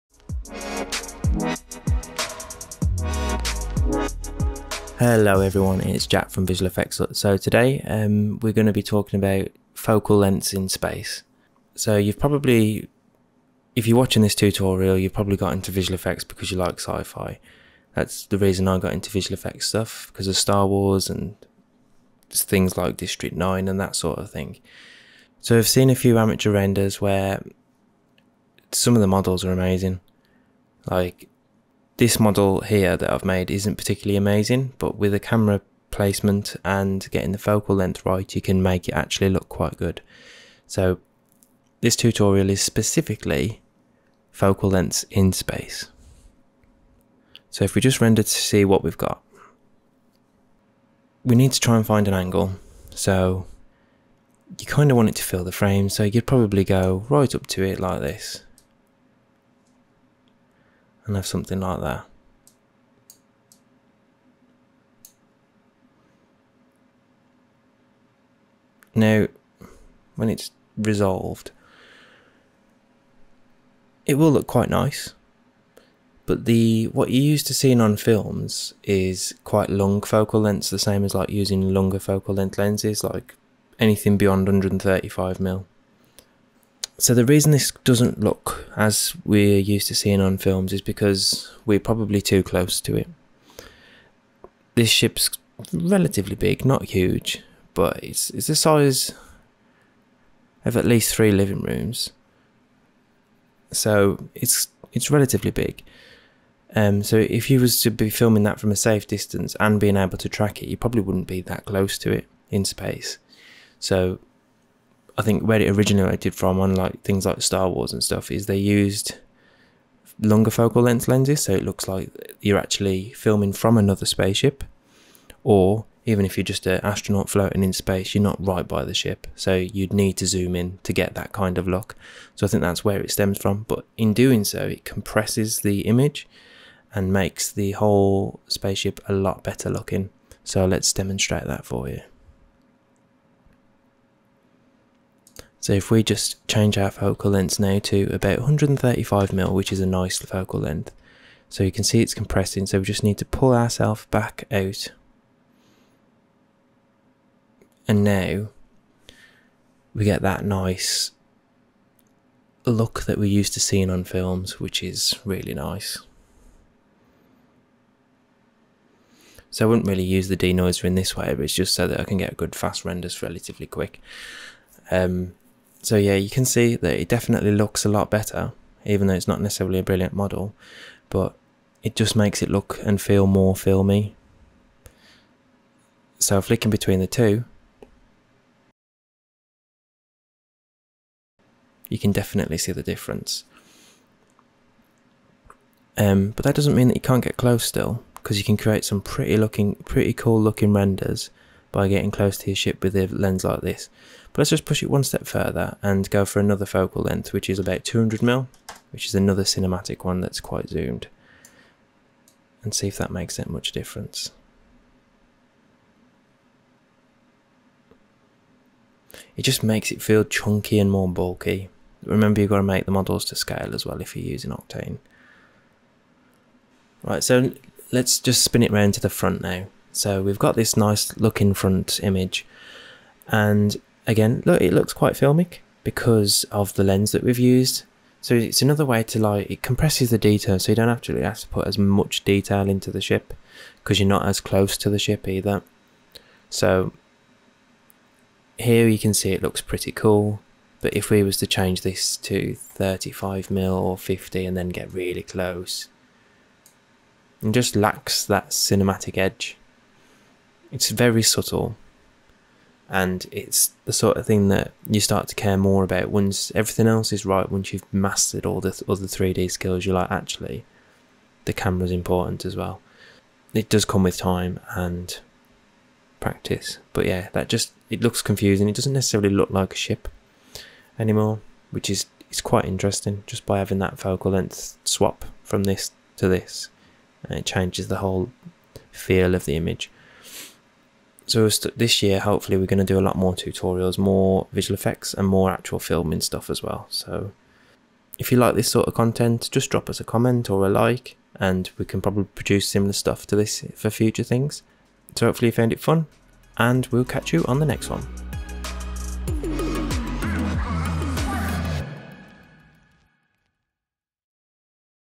3D Tips - Focal Lengths In Space (ft. Octane Render & Cinema 4D) | VFXHUT
In this video we will look at one camera setting that is really important for space scenes. In this demo we use Cinema 4D & Octane Render, but this setting is in all 3D software.

Take your 3D, VFX and Films to a higher level with premium assets for a lot less.